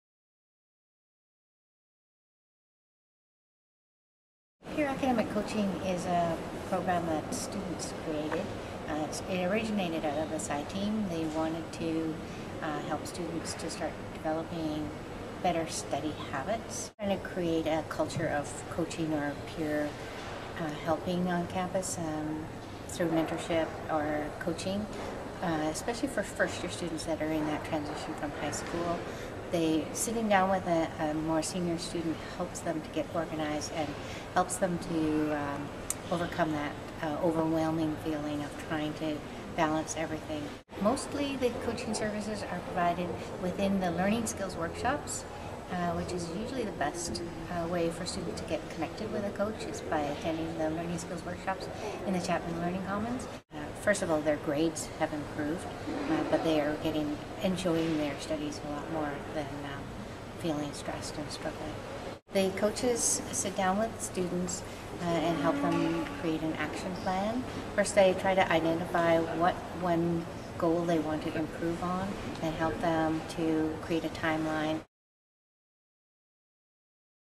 Peer Academic Coaching - How did it come about
Janet Sinclair talks about the value of Peer Academic Coaching and how it started.